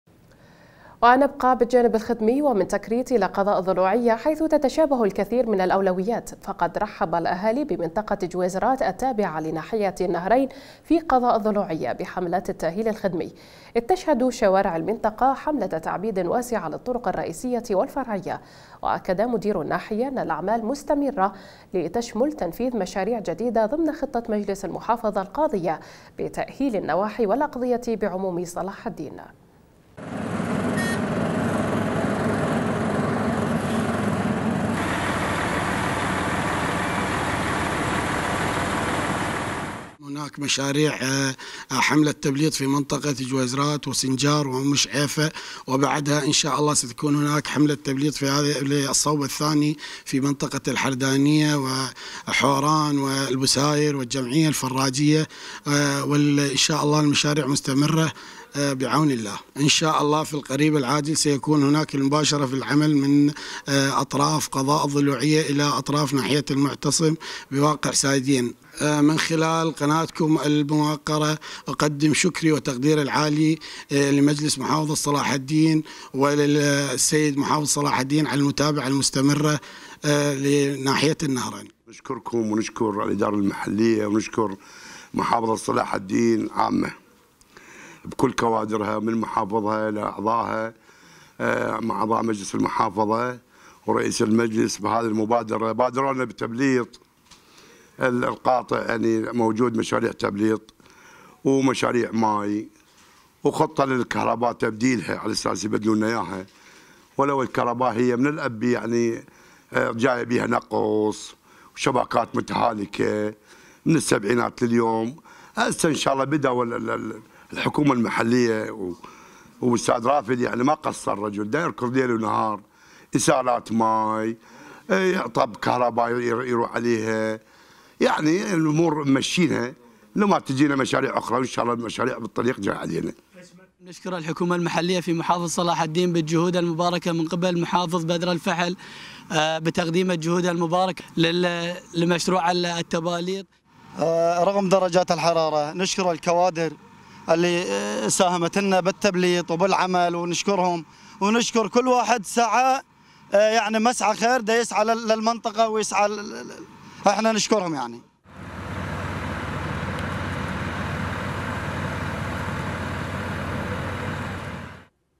ترحيب شعبي واسع بحملة تعبيد طرق جويزرات بالضلوعية
ونبقى بالجانب الخدمي ومن تكريت إلى قضاء الضلوعية حيث تتشابه الكثير من الأولويات؛ فقد رحب الأهالي بمنطقة جويزرات التابعة لناحية النهرين في قضاء الضلوعية بحملات التأهيل الخدمي؛ إذ تشهد شوارع المنطقة حملة تعبيد واسعة للطرق الرئيسية والفرعية، وأكد مدير الناحية أن الأعمال مستمرة لتشمل تنفيذ مشاريع جديدة ضمن خطة مجلس المحافظة القاضية بتأهيل النواحي والأقضية بعموم صلاح الدين.